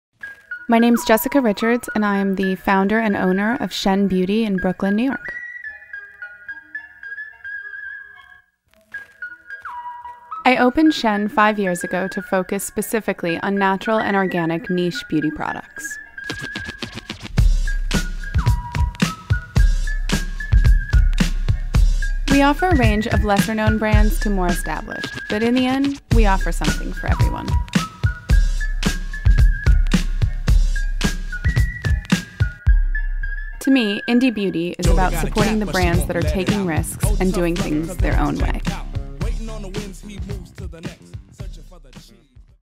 Jessica Richards of Shen Beauty for IBE
Shen founder, Jessica Richards is renowned in the beauty and fashion industry for her ability to find unique and luxurious niche brands including Amanda Lacey, Kate Logan, Percy & Reed, Aurelia, Ilia, Georgia Louise, Pursoma, Therapie Roques Oneil, and Suman to the American market. Formally at Vogue, has been featured in the New York Times, Vanity Fair, Interview, Allure, Harper’s Bazaar, The Huffington Post and Goop and currently serves as the Brand Strategist and Beauty Buyer for LuckyShops.com , as a Brand Advisor to ShopHQ/eVine and as a columnist to Yahoo Travel.

Indie Beauty Expo

The Altman Building
135 West 18th St., New York, NY